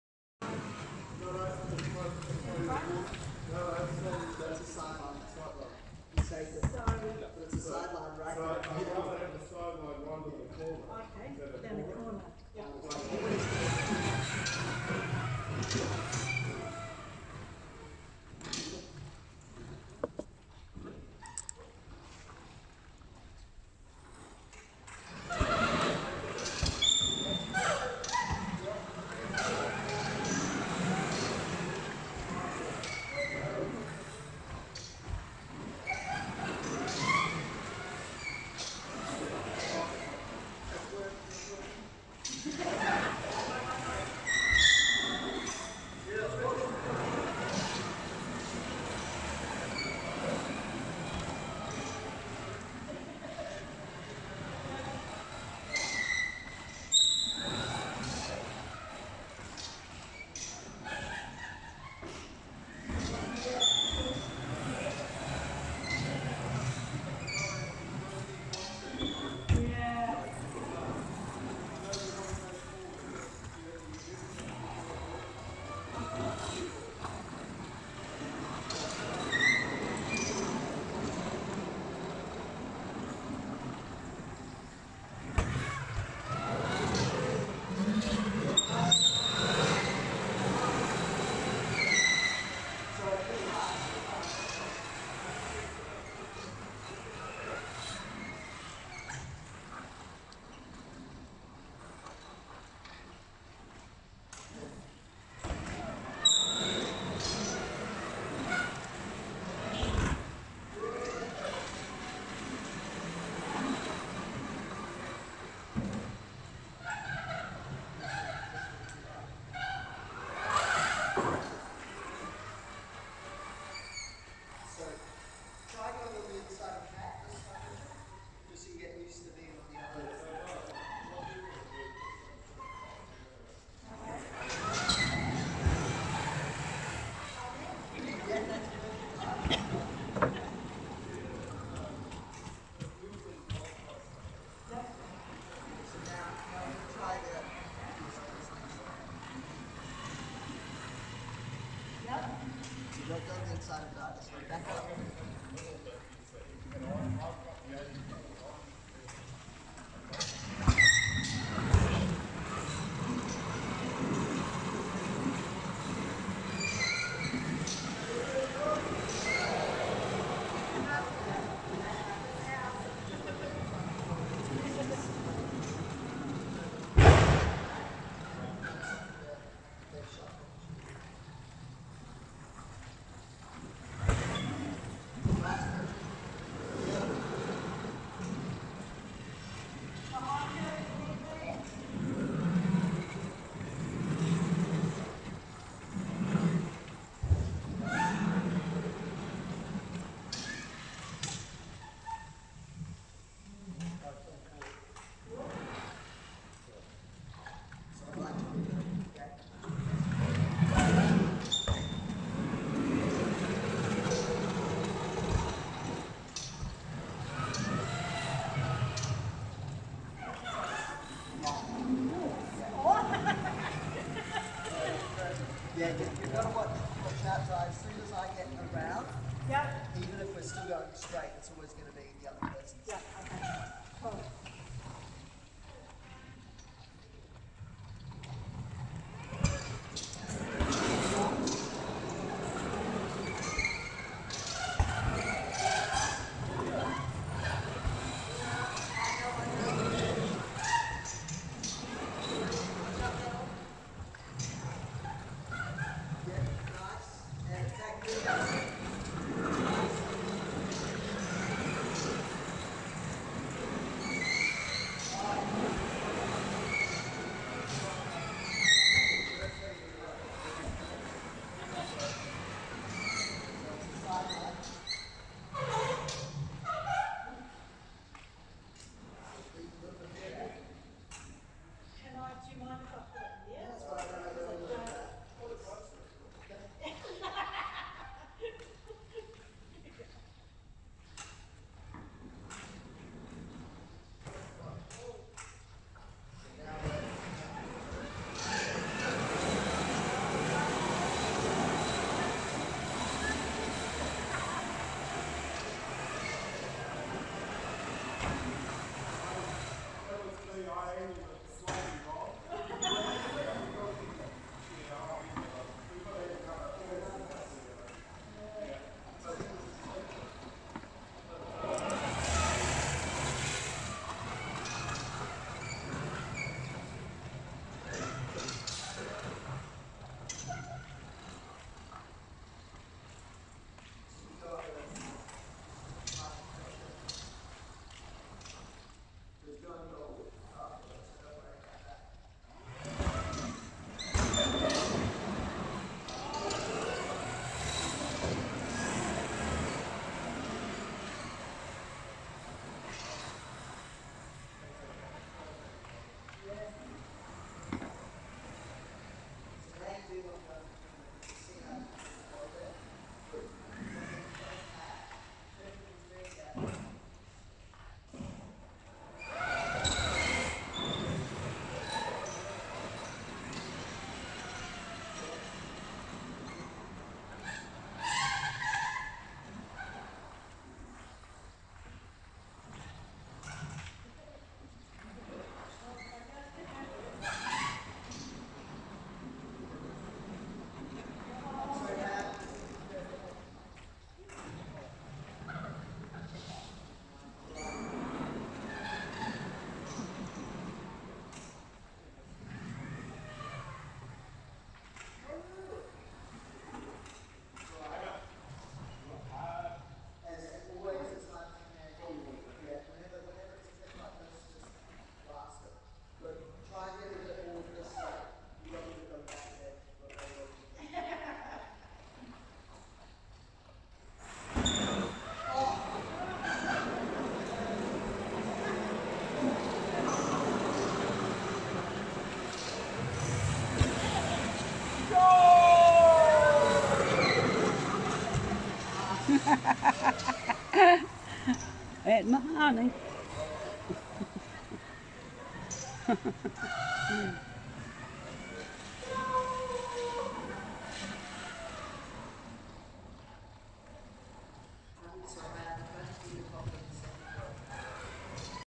Canterbury Powerchair Football
2 ON 2 practice working on skills. A bit different from full game practice but used for honing positioning skills